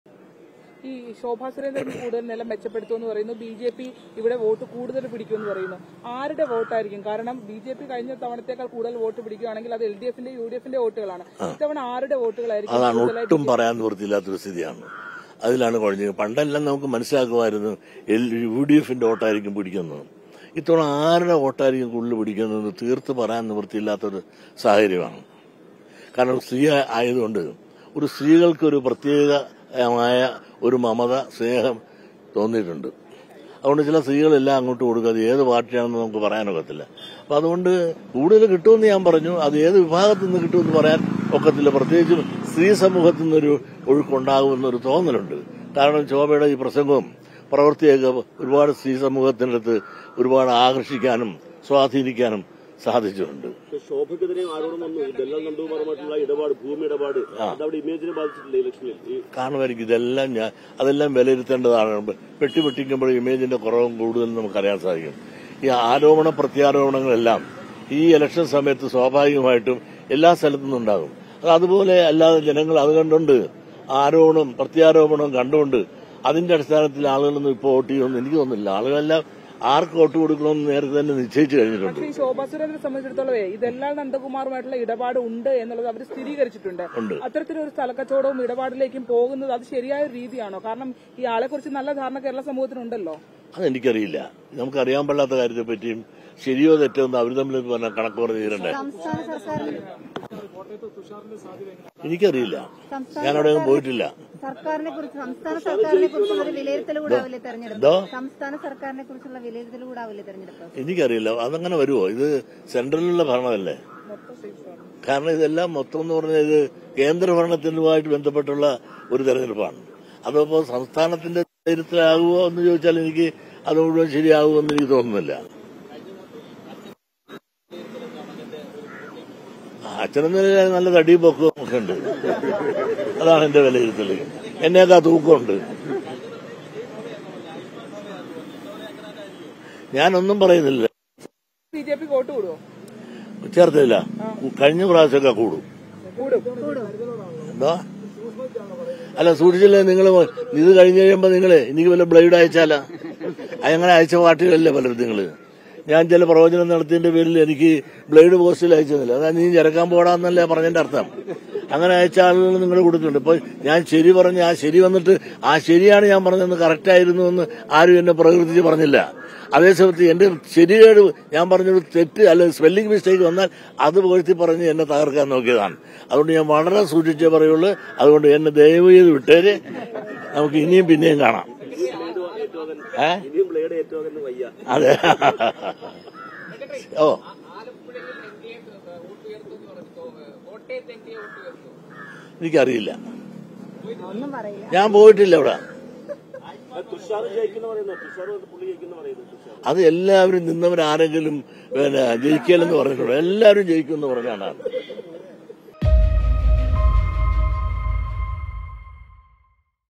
പ്രവചനം തെറ്റിയപ്പോള്‍, ചെരക്കാന്‍ പോടാന്ന് പറഞ്ഞ് ബ്ലേഡ് അയച്ചവരല്ലേ | Vellapally Natesan
'പ്രവചനം തെറ്റിയപ്പോള്‍ ചെരക്കാന്‍ പോടാന്ന് പറഞ്ഞതല്ലേ, ഇത്തവണ സൂക്ഷിച്ചേ പറയൂ, എന്നെ വിട്ടേര്'; വെള്ളാപ്പള്ളി നടേശന്‍ | Vellapally Natesan on Loksabha Election 2024

With Regards
Team RBC